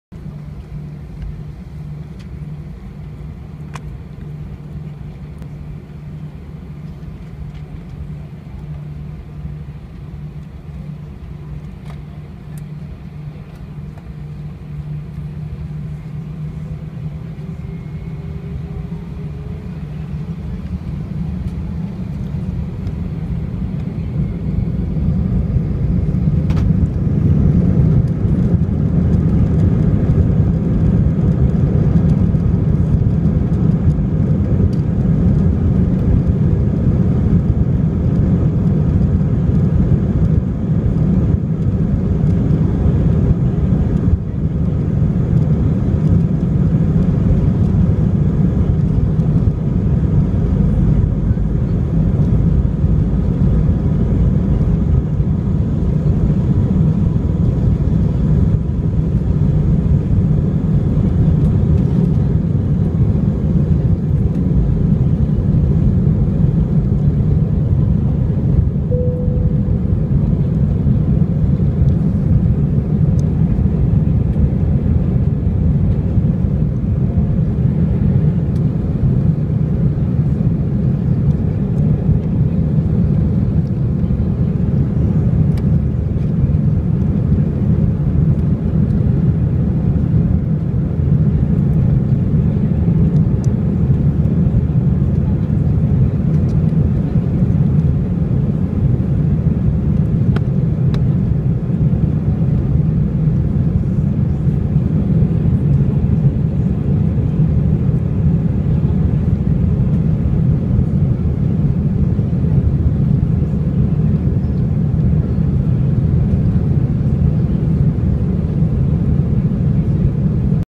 Spirit Airlines a319 take off from Chicago ORD
Spirit Airlines a319 take off from Chicago O'hare airport flight NK893 to Dallas DFW